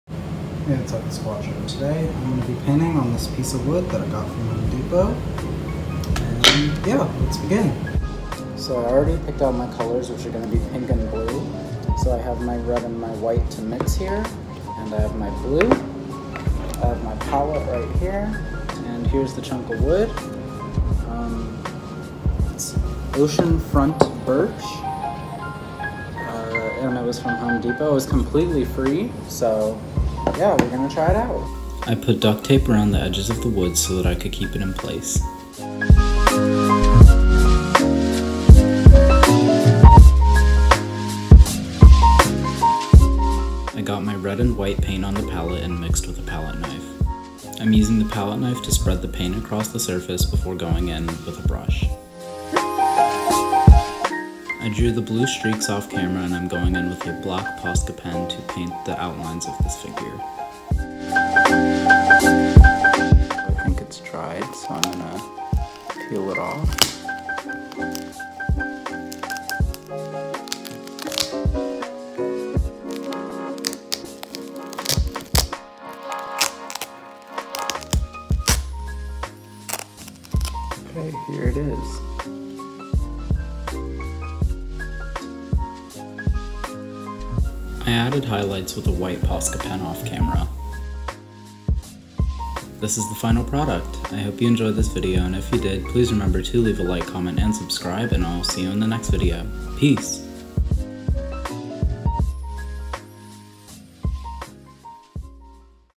Acrylic painting on wood
In this video I paint on a free wood sample from Home Depot (not sponsored)